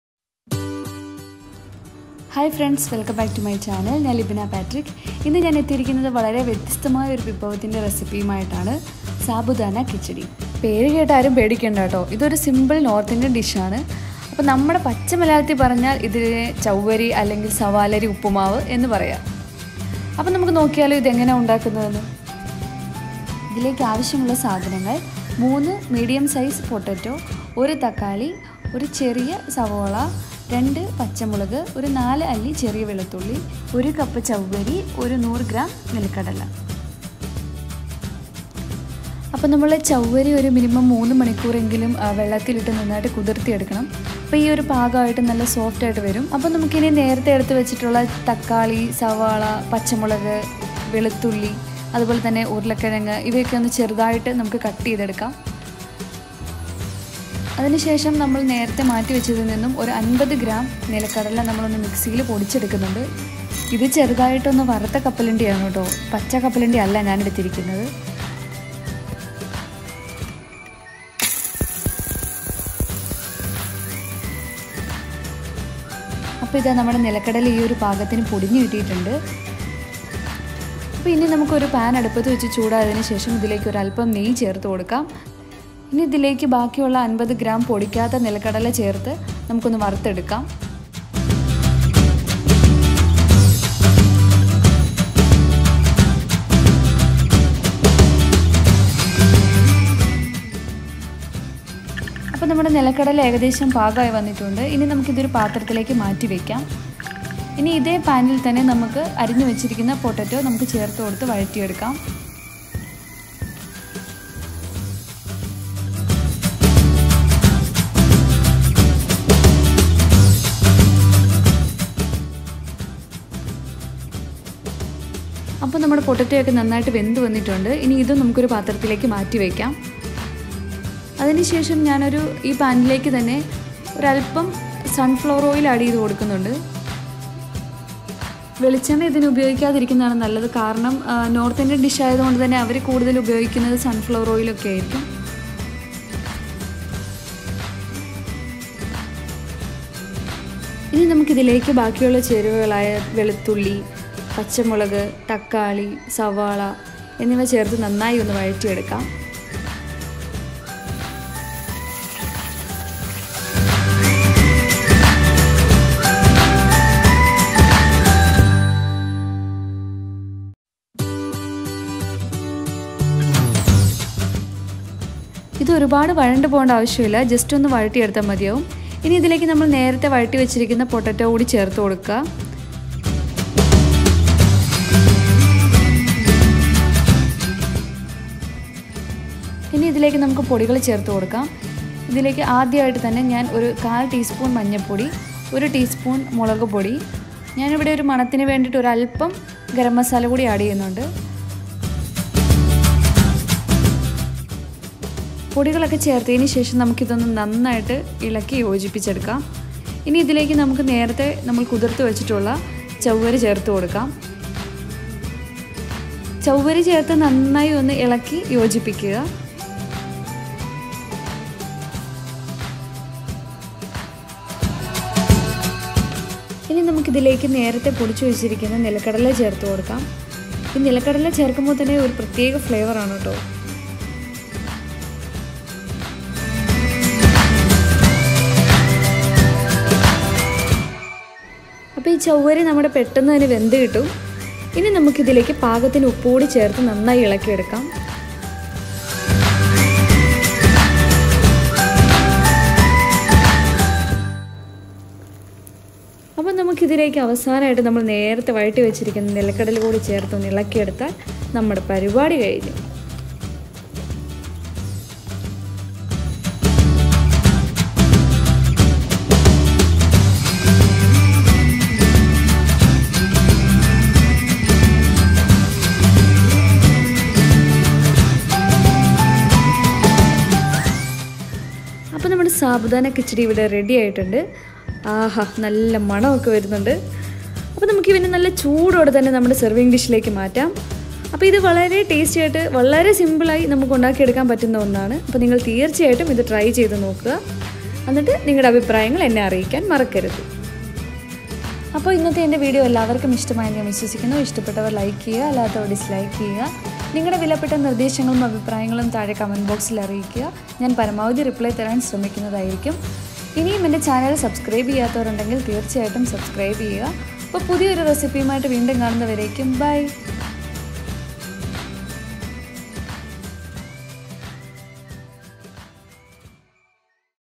മഞ്ഞുതുള്ളികൾ പോലെയൊരു ഉപ്പുമാവ് /Sabudana Upma/ Kichdi | sago upma recipe | ചവ്വരി ഉപ്പുമാവ്
INGREDIENTS
SAGO(CHOWARI) - 1 CUP
ONION - 1 SMALL/MEDIUM SIZE
GREEN CHILLY - 2 OR MORE
GARLIC- 4 PETALS
Peanut- 100 gm
SALT - NEEDFUL